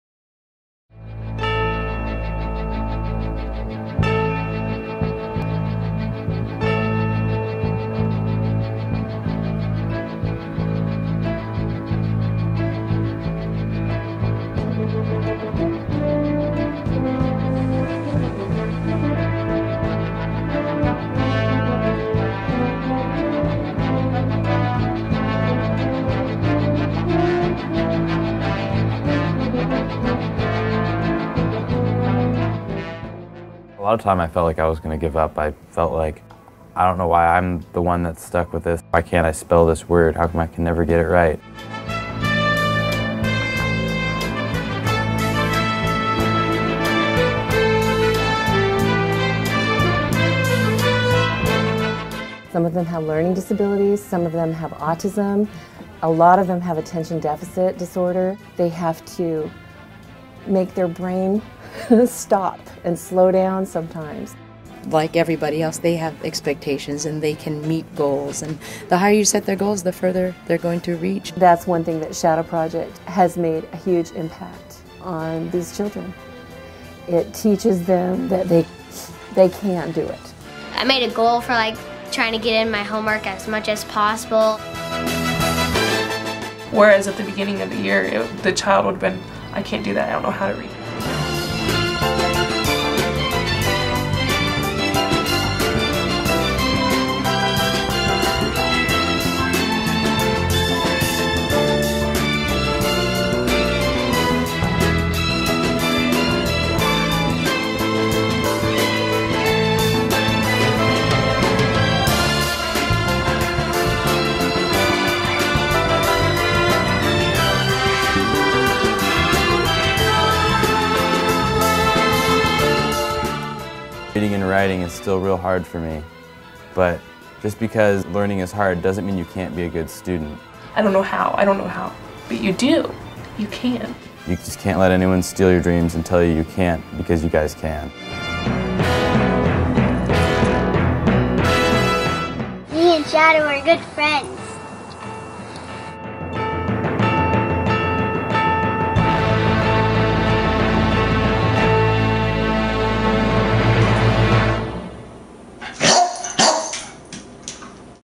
The Shadow Project Give!Guide 2012
Inspired by a child. Named for his dog. The 'Shadow' Project is a Portland nonprofit that teams with teachers in 28 schools to help kids with special needs discover the joy of learning and turn "I can't" into "I can!" Give $100 or more and The Miller Foundation will match your donation dollar for dollar!